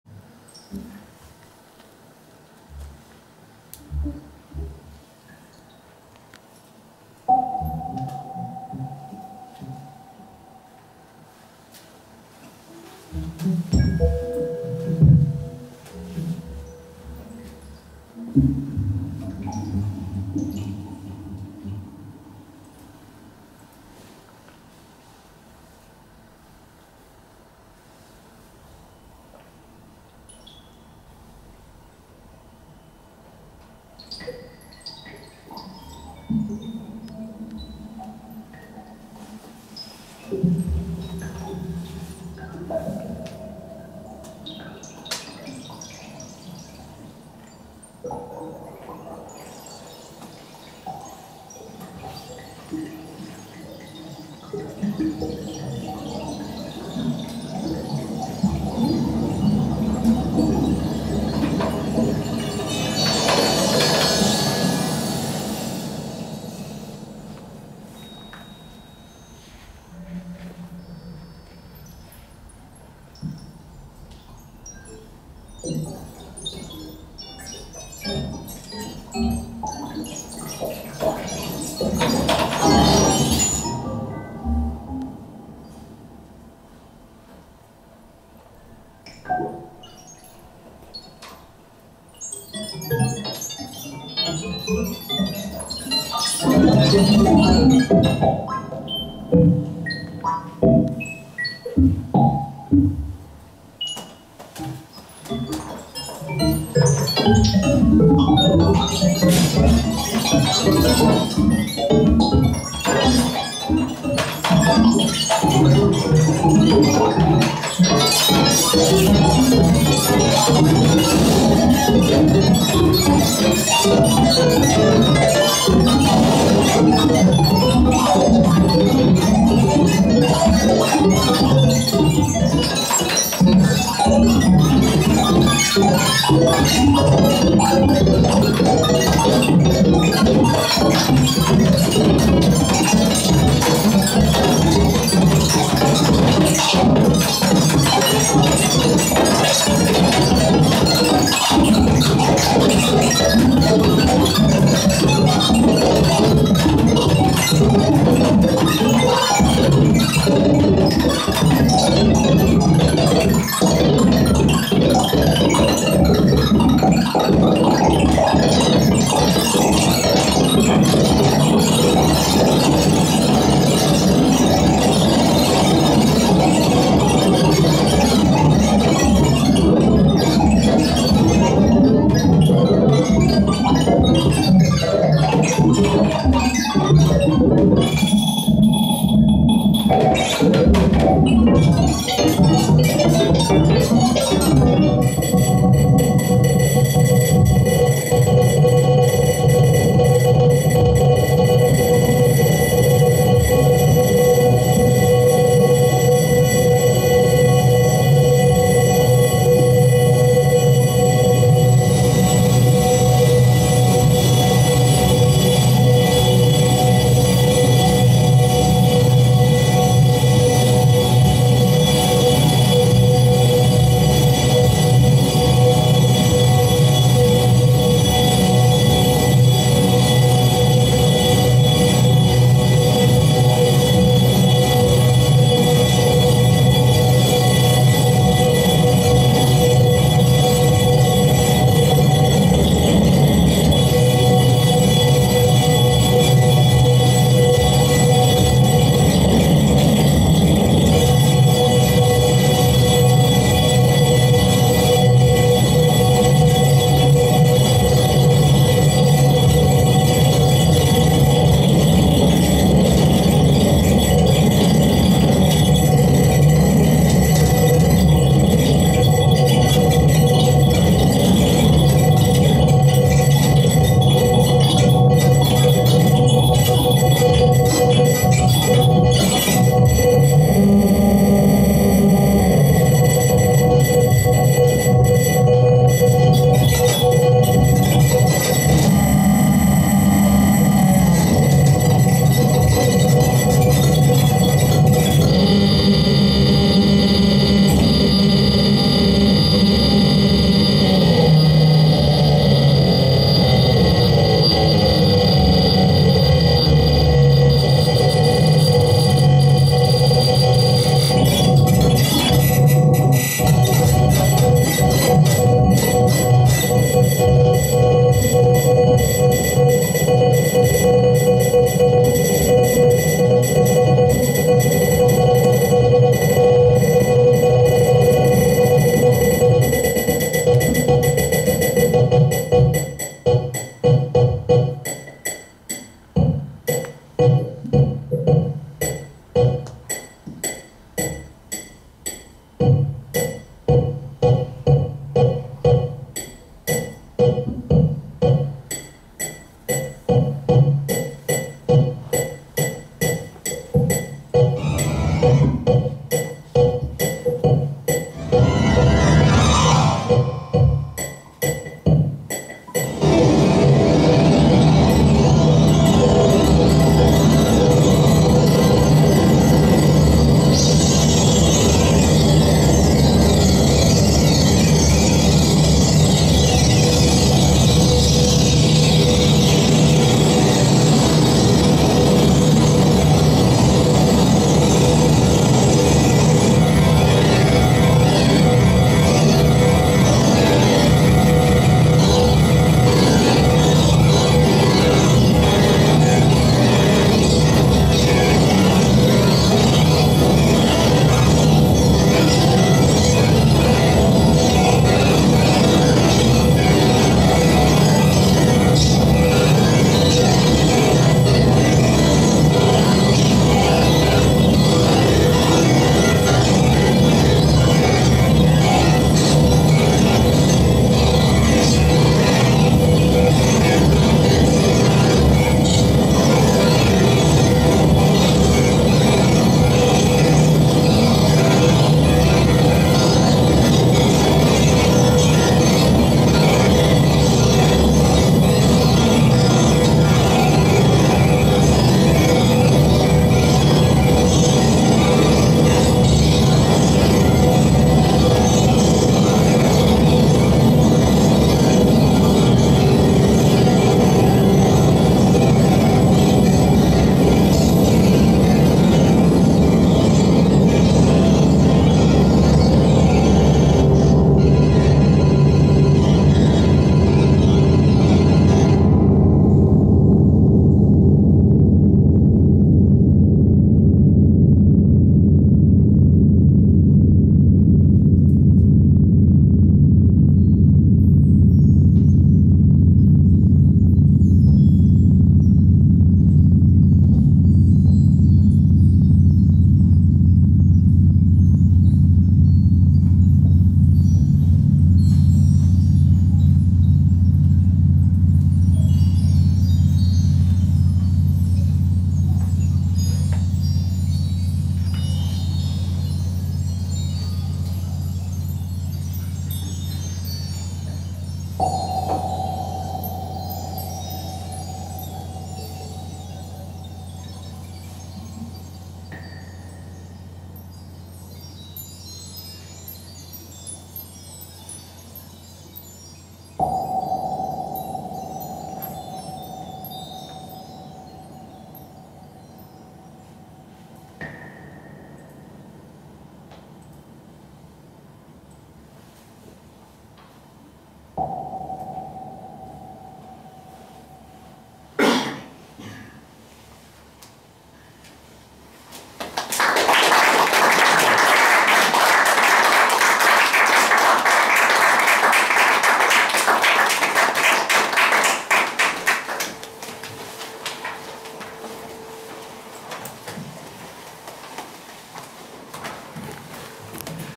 Gintas K live at ICMC-NYCEMF, New York City 2019
Gintas K live  at The International Computer Music Conference (ICMC) and New York City Electroacoustic Music Festival (NYCEMF), Fridman Gallery , New York, USA 2019